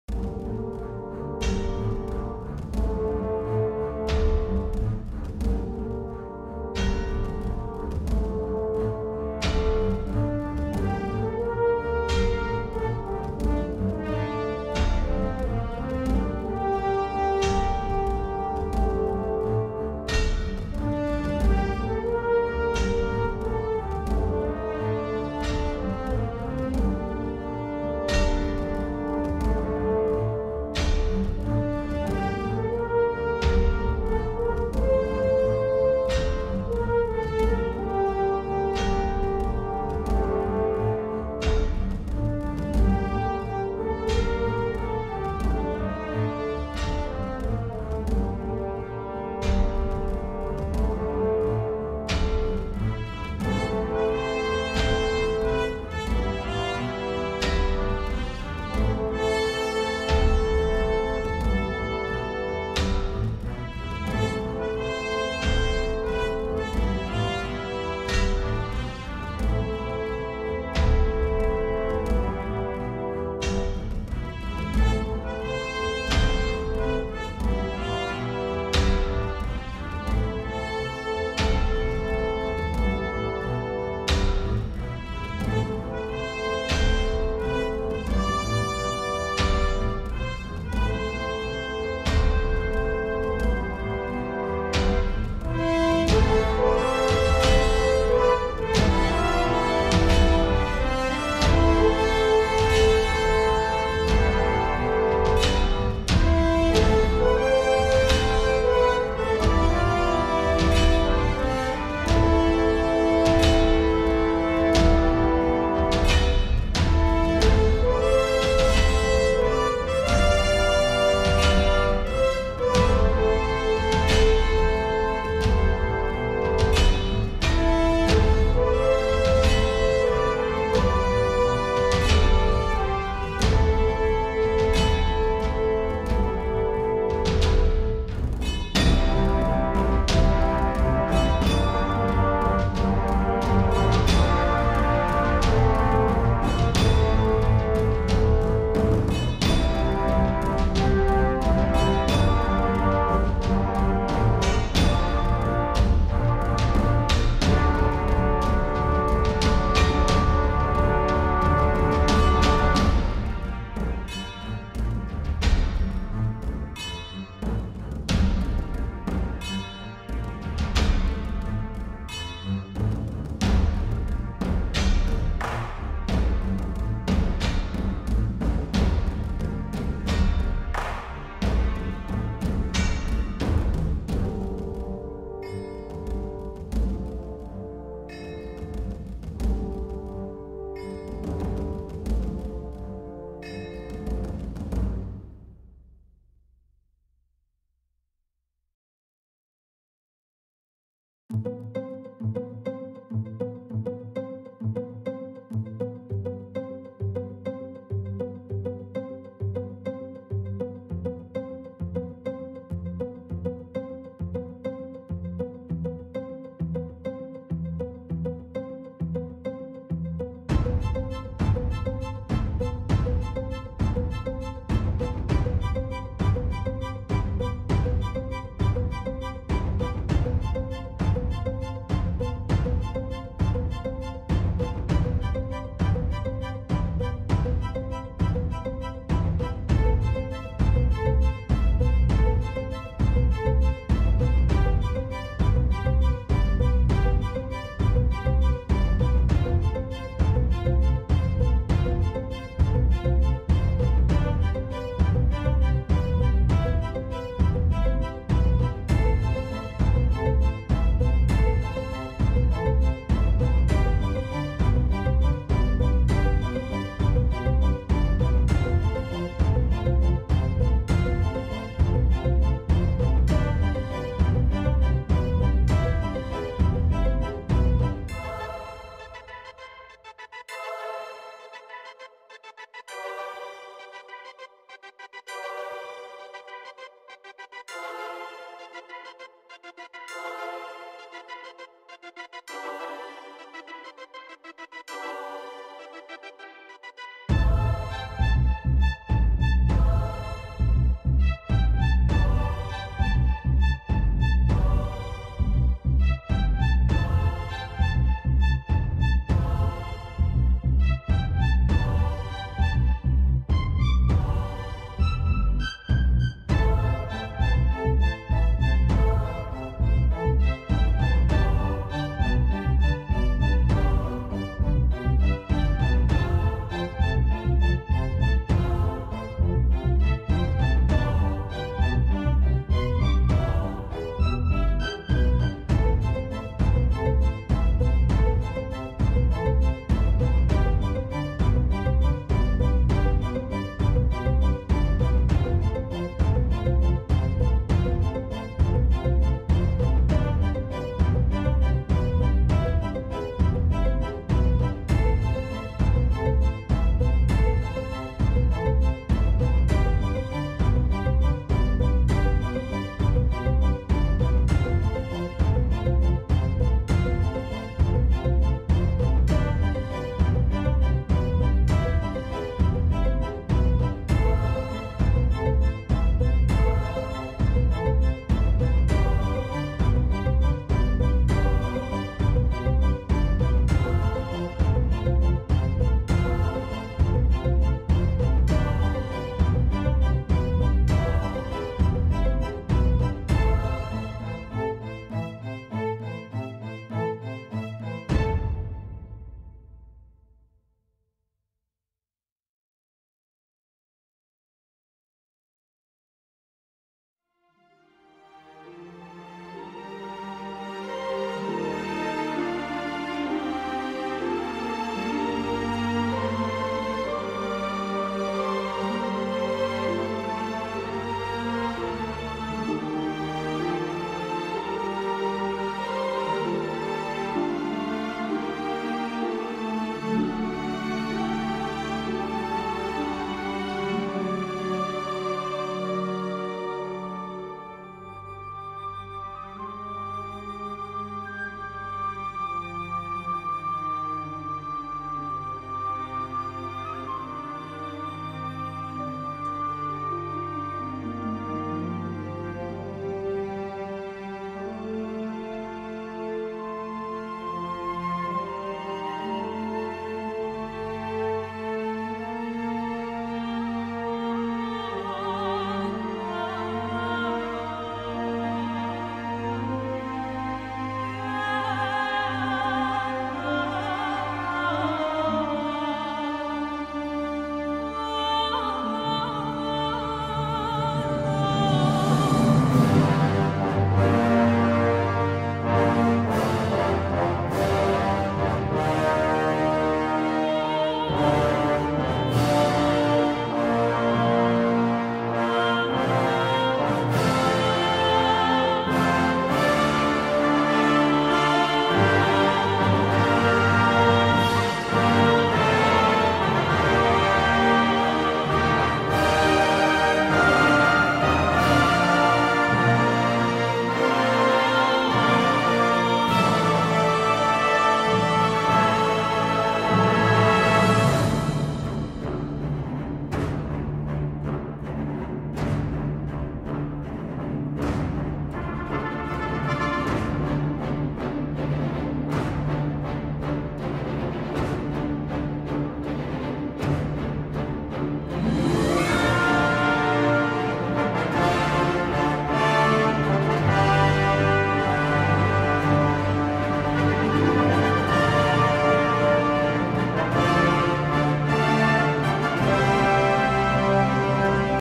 The crusades of the Eastern Mediterranean : every day/other day/3 days/5 days/month
Errors: Cyprus should be shown separately from Acre, Georgia should be c have "seljuks" removed in 1124, the Turks controlled the Cillian plains in 1096, Ahlat was conquered in 1207, i accidently put it back in 1218, Marqab fell in 1285, not in 1268.
Sources: i found 4 books, 3 by Steven Runciman, 1 by Wisconsin University press. Note that Steven Runciman is biased however. You can download them at Bo ok.org by typing crusades into the search bar